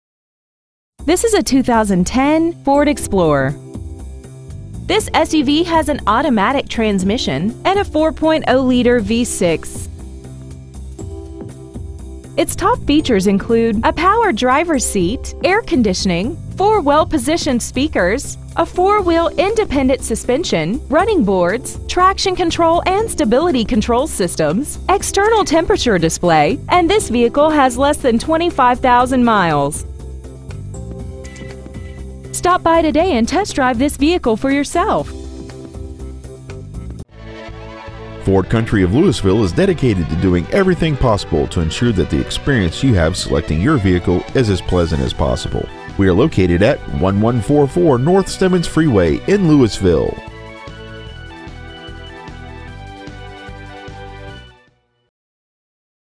2010 FORD EXPLORER Lewisville TX
Year : 2010
 Make : FORD
 Model : EXPLORER
 Engine : 4.0L V6
 Trans . : AUTO
 Exterior : BLACK
 Miles : 24,846
 Interior : CAMEL
 Stock : P4460